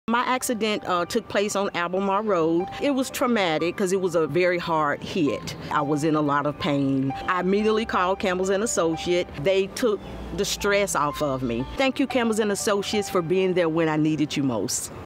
Auto Accident Client Testimonial | Campbell & Associates
Angela's client testimonial about her experience with Campbell & Associates